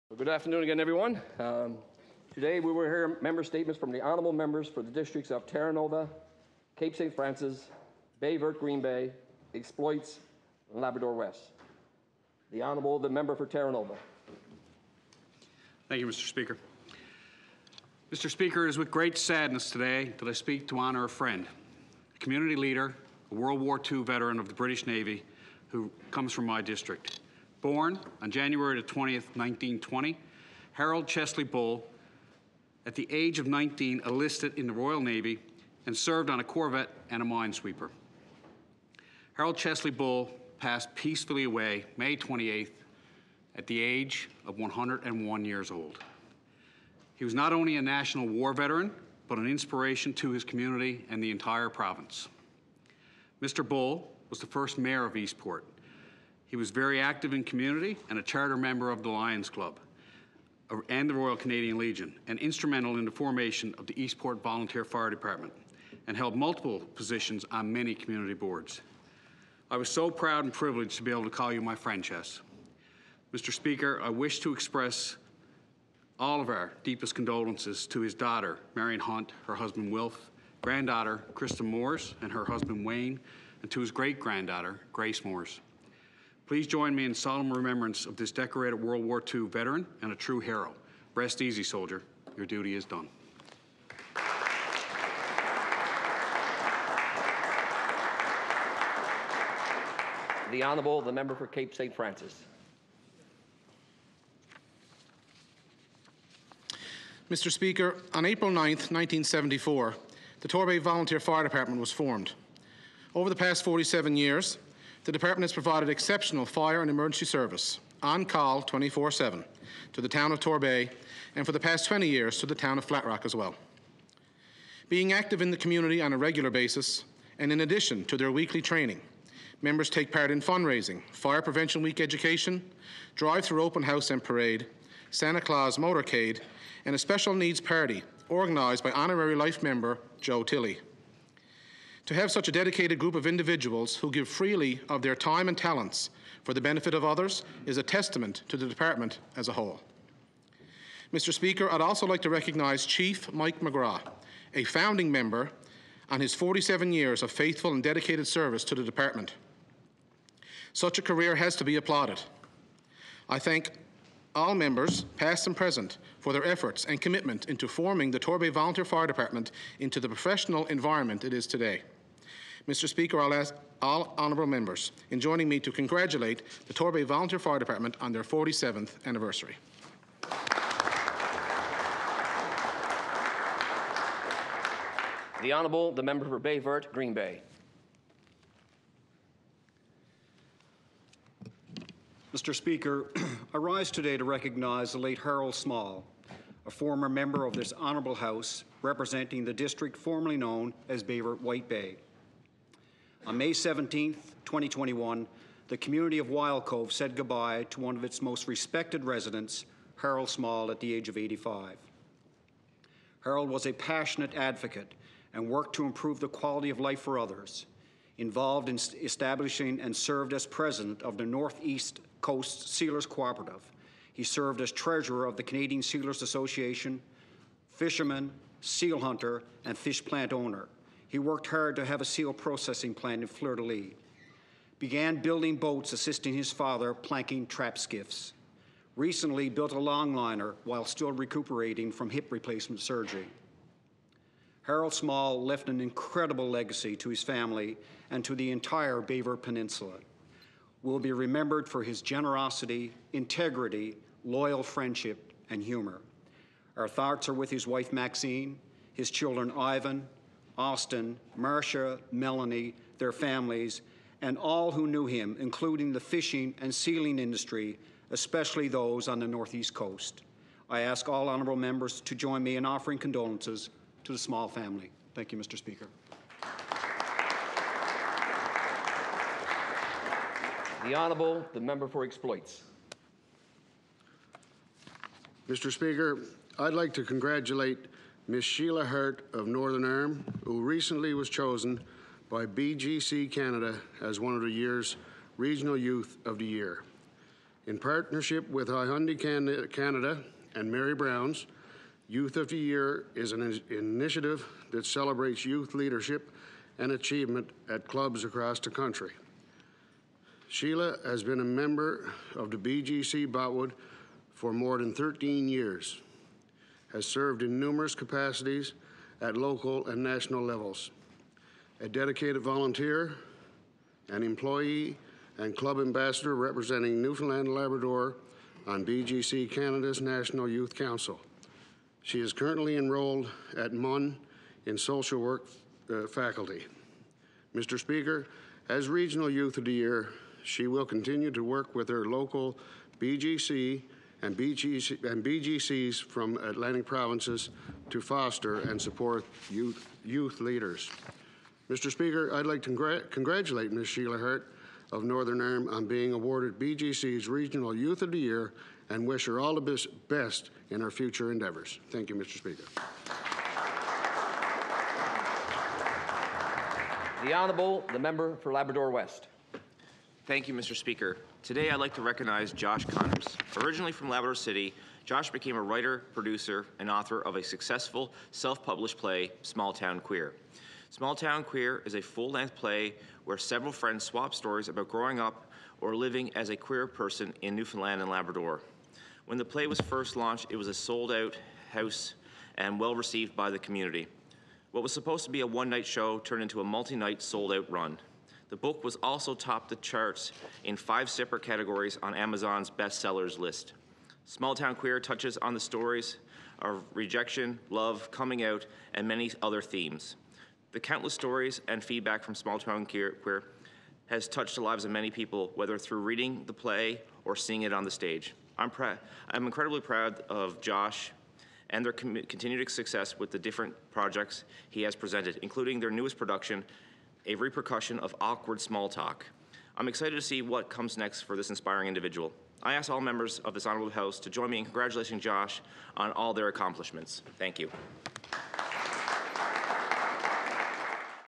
Members' Statements - NL House of Assembly - June 1 2021
Members' Statements delivered in the Newfoundland & Labrador House of Assembly.